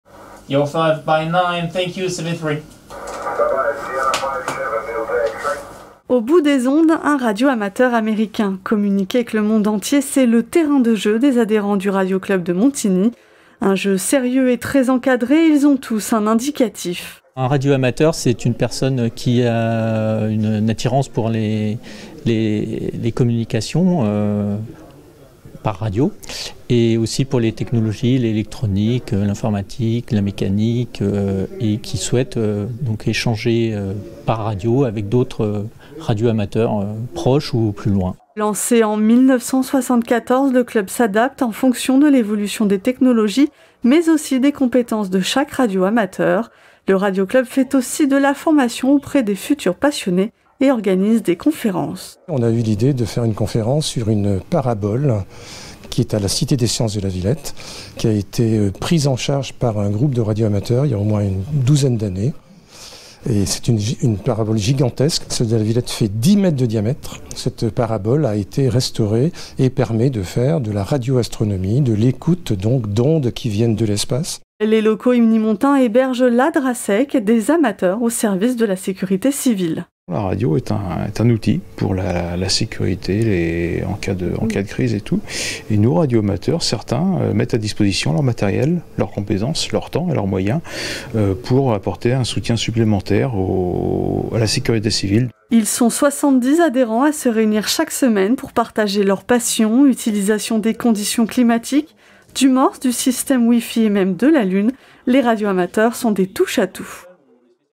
Yvelines | Au radio-club de Saint-Quentin-en-Yvelines, les ondes rassemblent les passionnés
Difficile d’imaginer qu’à Montigny-le-Bretonneux, deux fois par semaine, des passionnés de radio communiquent avec le reste du monde. Situé sur la commune depuis une quinzaine d’année, le radio-club de Saint-Quentin-en-Yvelines fait plus que tenter de communiquer : il crée des liaisons radio haute-fréquences, joue avec son environnement et peut même être un soutien à la préfecture. Il nous a accueilli.